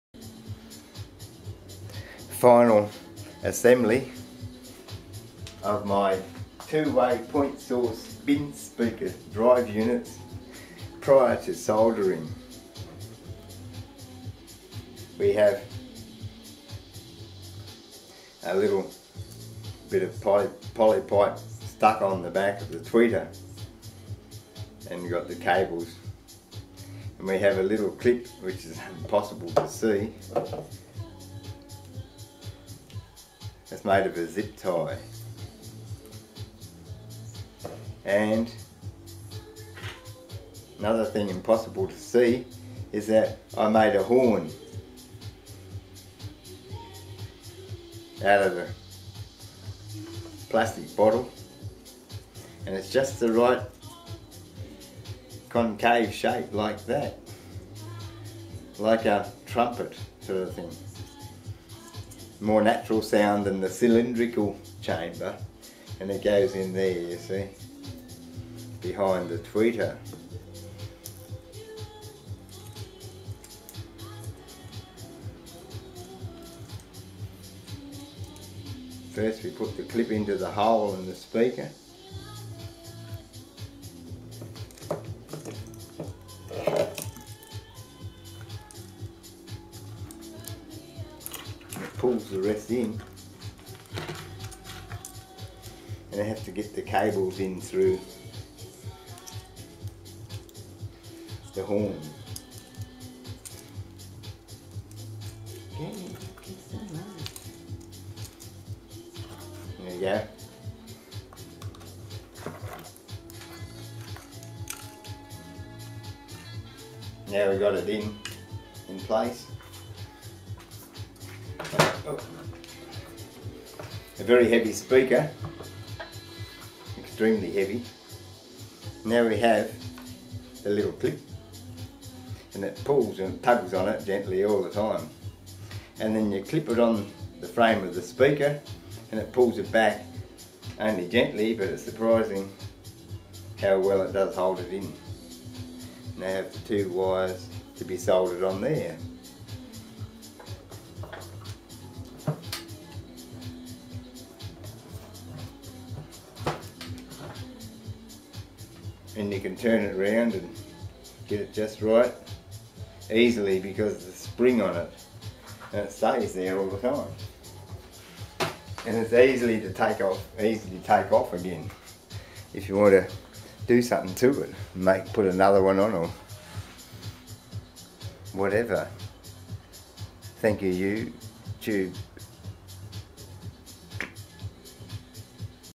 ASSEMBLY OF POINT-SOURCE TWO WAY SPEAKERS (COIL TWEETER).
The bass-med driver is a 10" "bin-speaker" (150 W power handling). It is also very efficient (compared with the small tweeter) so the bass-med driver is held back with a resistor. Even with this, these still have more output (per watt) than my (point-source) 6" lounge hi-fi speakers. They are also on you tube, on a video called "My Point-Source Speaker Workshop" and "Weird Looking Speakers".
The speakers on this video are also on You Tube "(Jazz) Point-Source Speaker Demo (coil-tweeter).